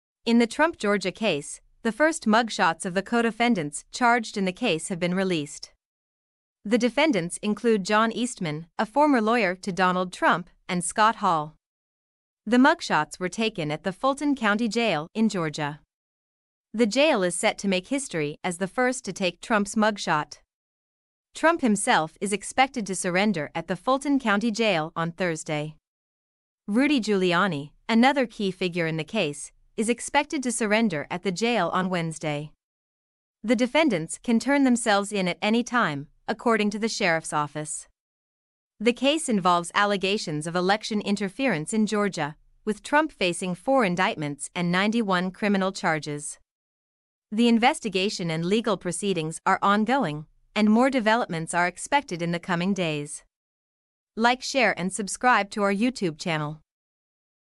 Trump Georgia case First mugshots released, Giuliani could surrender tomorrow
In the Trump Georgia case, the first mugshots of the codefendants charged in the case have been released. The defendants include John Eastman, a former lawyer to Donald Trump, and Scott Hall. The mugshots were taken at the Fulton County Jail in Georgia. The jail is set to make history as the first to take Trump's mugshot. Trump himself is expected to surrender at the Fulton County Jail on Thursday. Rudy Giuliani, another key figure in the case, is expected to surrender at the jail on Wednesday. The defendants can turn themselves in at any time, according to the sheriff's office. The case involves allegations of election interference in Georgia, with Trump facing four indictments and 91 criminal charges. The investigation and legal proceedings are ongoing, and more developments are expected in the coming days.
(Mehwish Mukhtar)